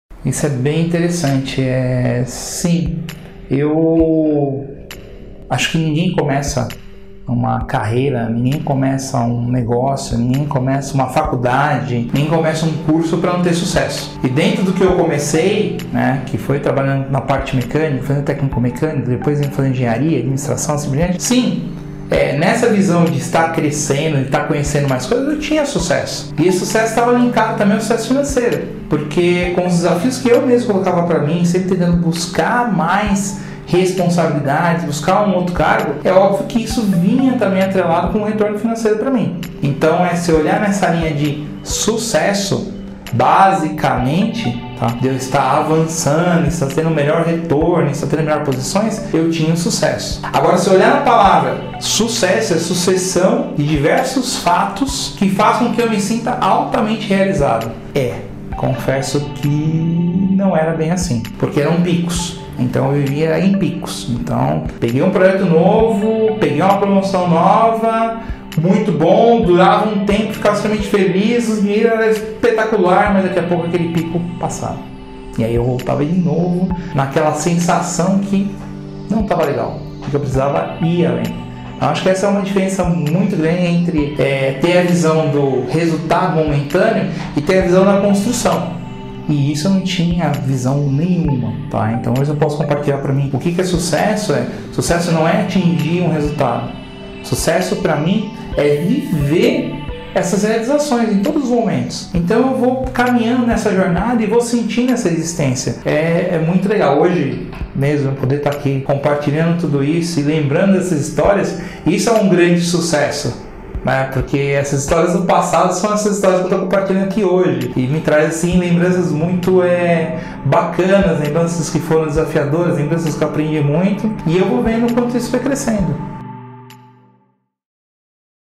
Sucesso Ou Realização? - O Que Não Te Contaram
No EP. 3 da série "Revelações Que Não Te Contam", você vai descobrir desafios, situações e revelações sobre carreira e negócios que vão ajudar você a entender as 03 esferas da vida (pessoal, profissional e financeira). Você vai entender como a diferença entre sucesso e realização pode afetar na sua vida pessoal, profissional e na sua saúde financeira através de experiências práticas de carreira e vida.

Quer saber mais? Acesse o blog